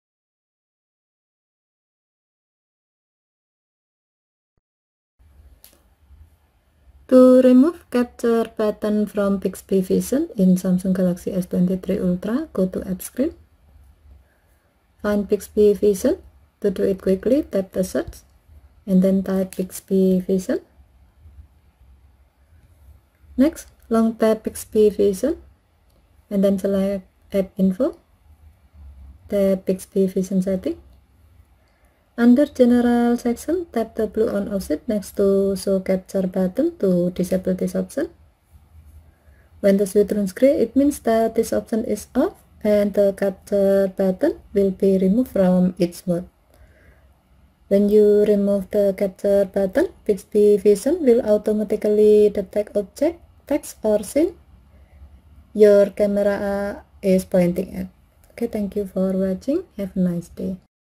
Samsung Galaxy S23 Ultra : How to remove the capture button from Bixby vision
In this video, you will find the steps on how to remove the capture button from Bixby vision in Samsung Galaxy S23/S23+/S23 Ultra. You can do so by disabling the Show capture button option within Bixby vision settings. When this option is disabled, the capture button has been removed from each mode, except in the scene describer screen. When you remove the capture button, Bixby vision will automatically detect object, text, or scene you're pointing at. This video uses Samsung Galaxy S23 Ultra SM-S918B/DS International version runs with Android 13(Tiramisu)